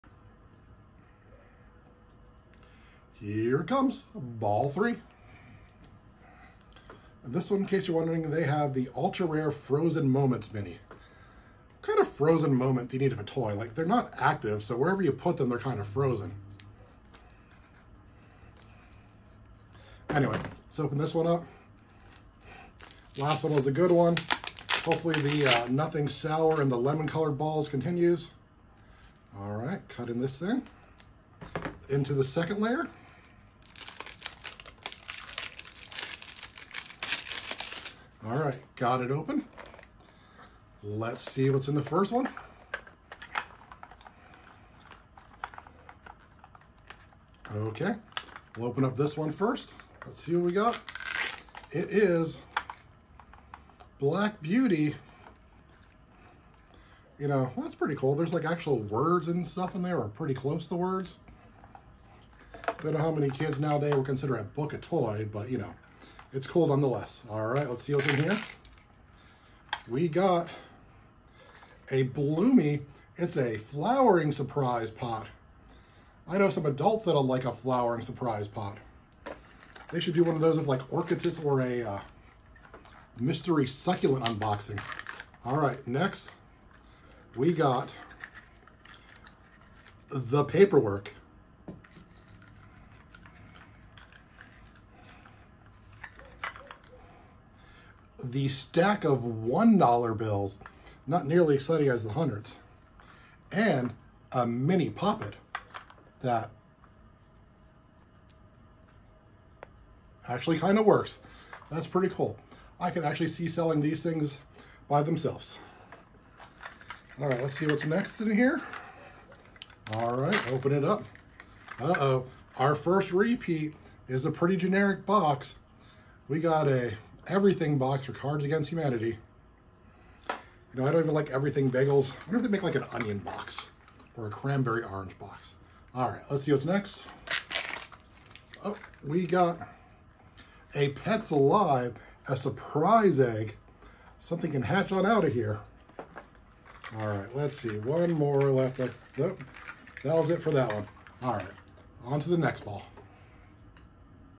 Toy Mini Brands! Series 3 Part 3
Went to Target and got some of the new Toy Mini Brands Series 3